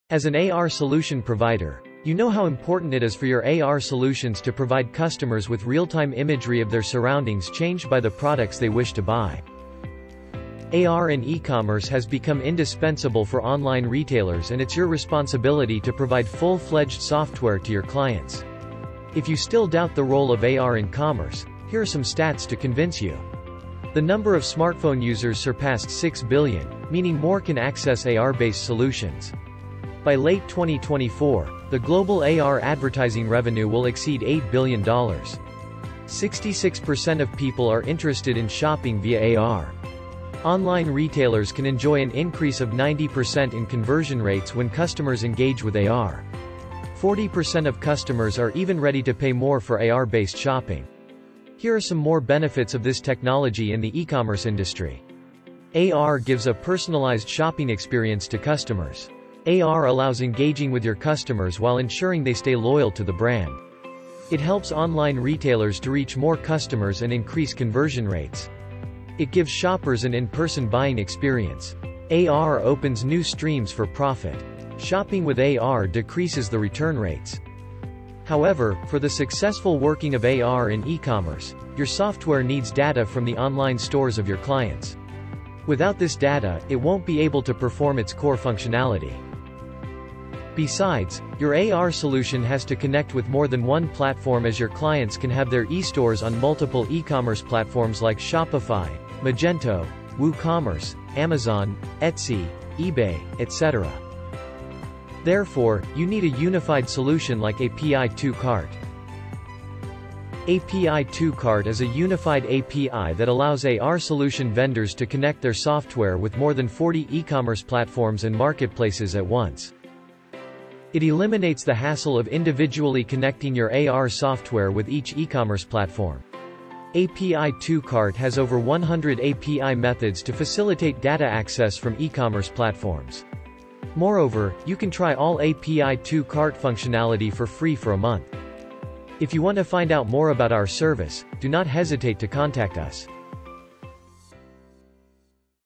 AR in eCommerce: How to Integrate with eCommerce Platforms
AR in eCommerce has become indispensable for online retailers and it’s your responsibility to provide full-fledged software to your clients.

However, for the successful working of AR in eCommerce, your software needs data from the online stores of your clients. Besides, your AR solution has to connect with more than one platform, as your clients can have their e-stores on multiple eCommerce platforms like Shopify, Magento, WooCommerce, Amazon, Etsy, eBay, etc.

API2Cart is a unified API that allows AR solution vendors to connect their software with more than 40 eCommerce platforms and marketplaces at once.

Find out about the role of AR in eCommerce and discover how AR solutions can easily integrate with popular shopping platforms